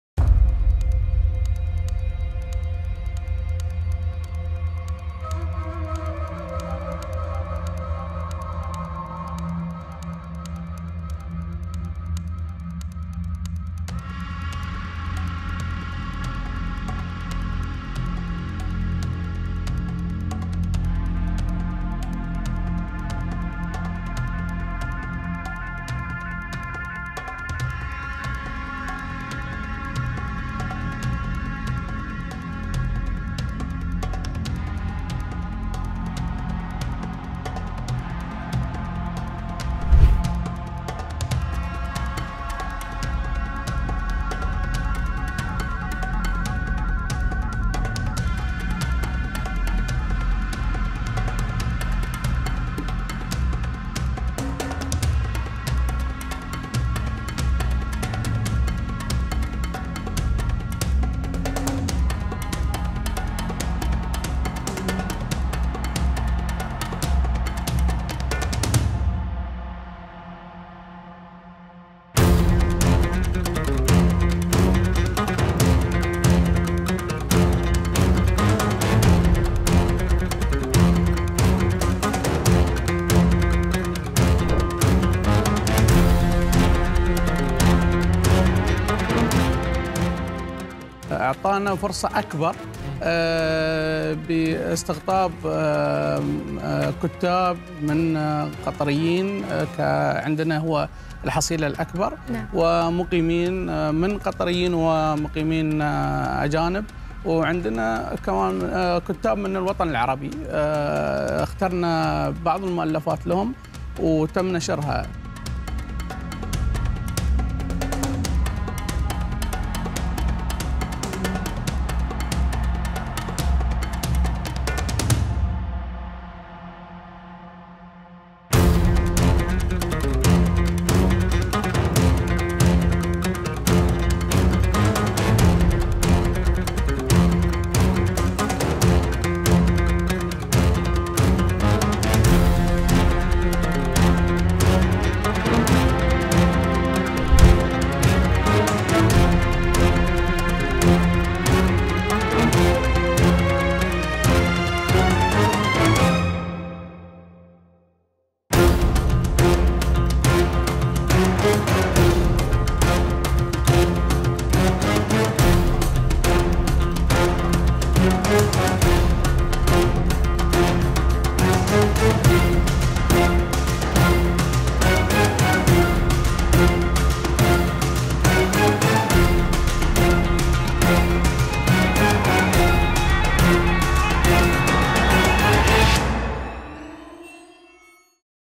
قطر حاضنة الإرهاب والتطرف.. الراعي الرسمي لمعرض الكتاب العربي بأوروبا
بعد نحو أسبوعين من تنظيمها لمعرض كتاب رقمي في السويد شمال أوروبا، تعتزم قطر تنظيم معرض آخر 5 أكتوبر/تشرين أول الجاري في الدنمارك
وتنشط قطر بإقامة تلك الفعاليات الثقافية المتواصلة بالتزامن مع تمويلها مراكز إسلامية ومؤسسات تعليمية في الغرب
وحذر مراقبون من مخطط قطري لاختراق الغرب، ولا سيما شمال أوروبا، ثقافيا عبر تمويلات مشبوهة لتلك الفعاليات الثقافية والمراكز الإسلامية والتعليمية، لنشر أفكار الجماعات الإرهابية التي تدعمها، وعلى رأسها تنظيم الإخوان